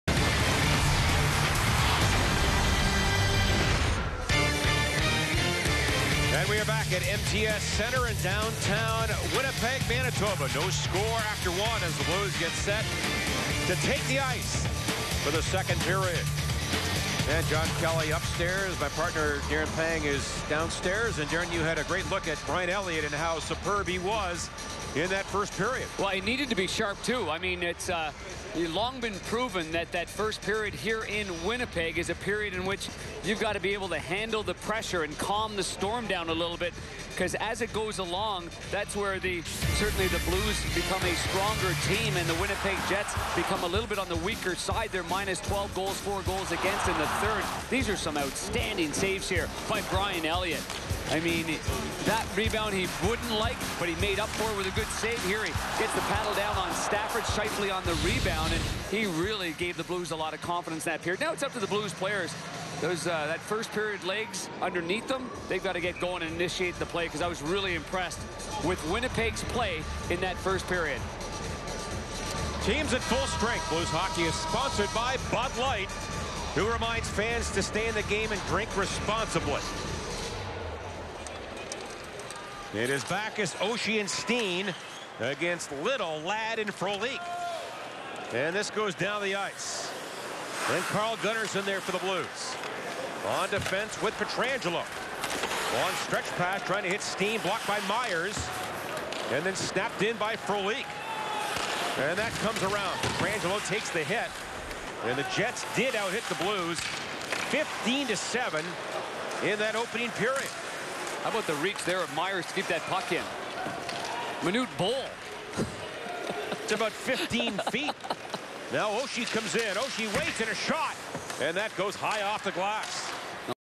Brian Elliott saves in 1st period. St. Louis Blues vs Winnipeg Jets Feb 26 2015 NHL
PBP John Kelly, color Darren Panger. FSMW feed.